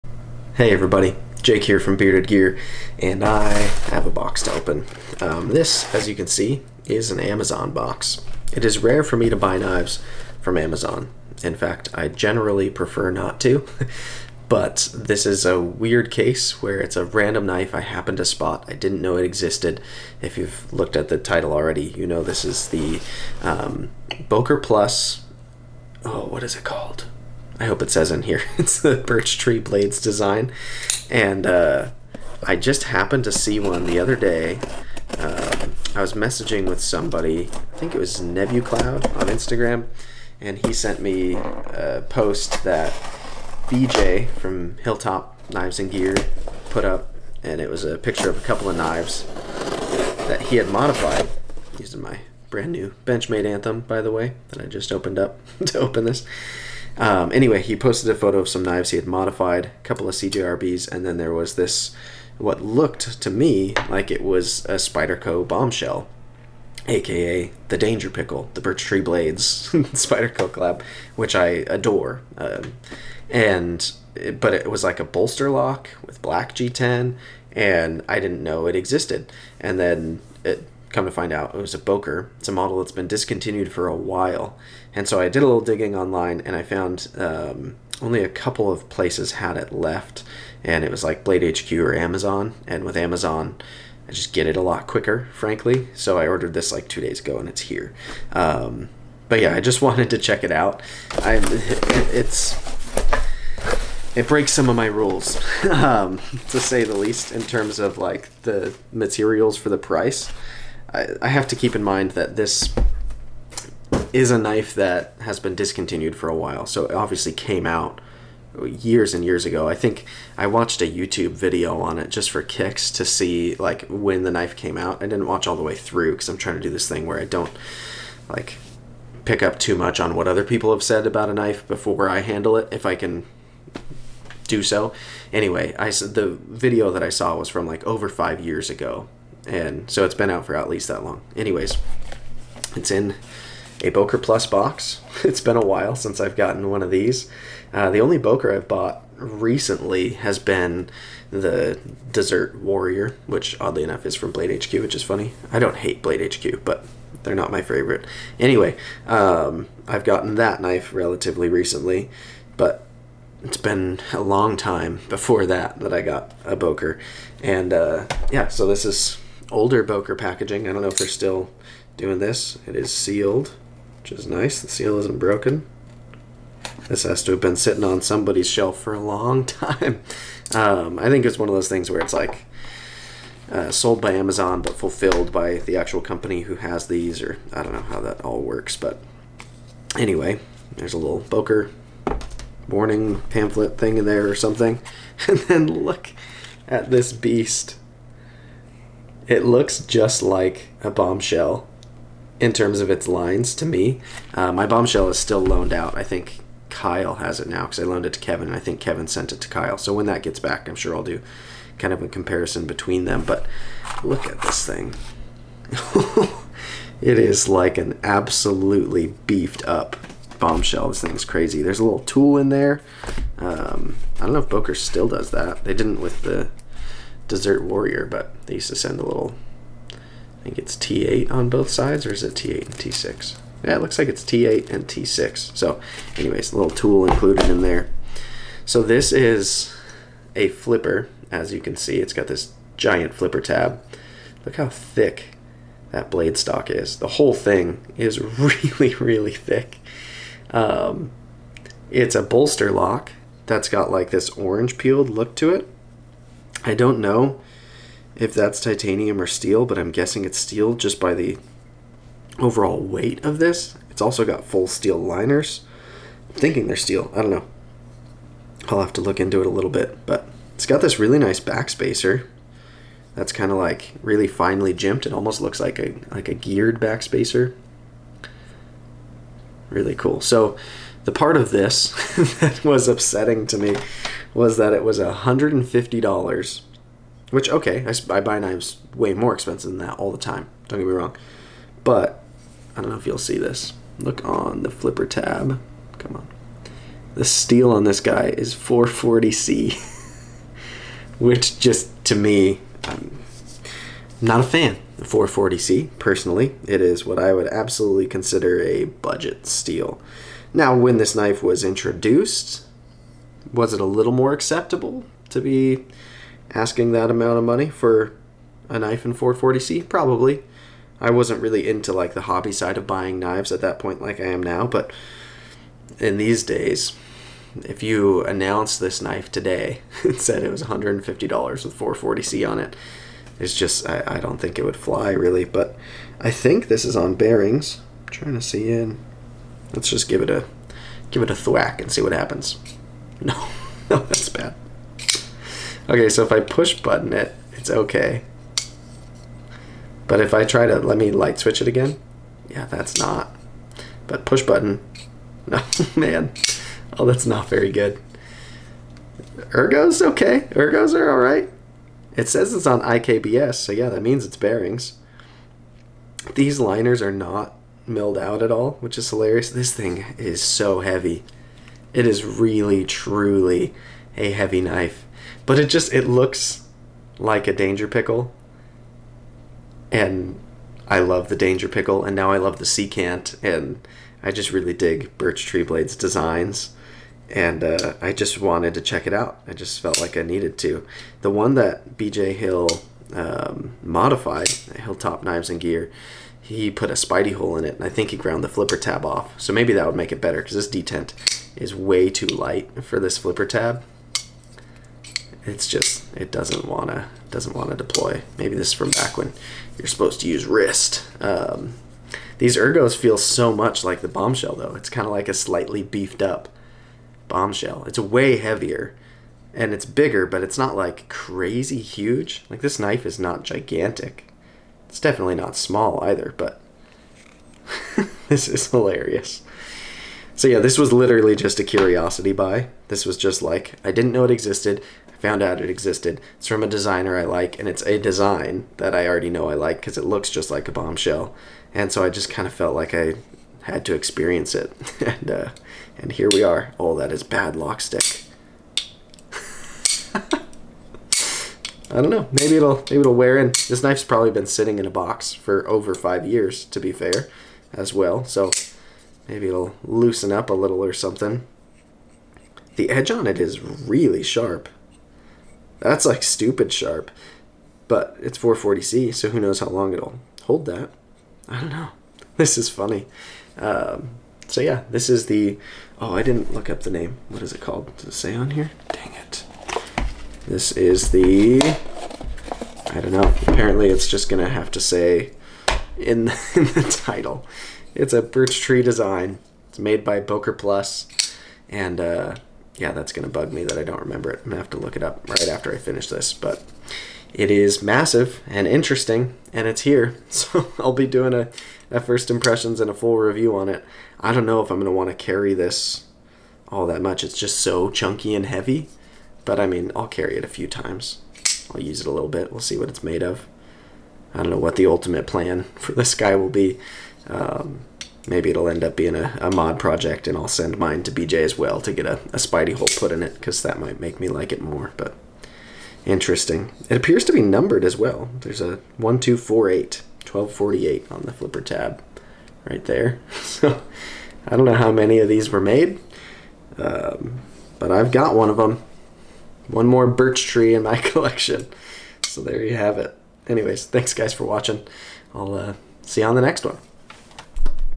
Boker Impetus Unboxing! Another Burchtree Blades Design!
In this video I unbox my new Boker Impetus which was designed by Michael Burch of Burchtree Blades!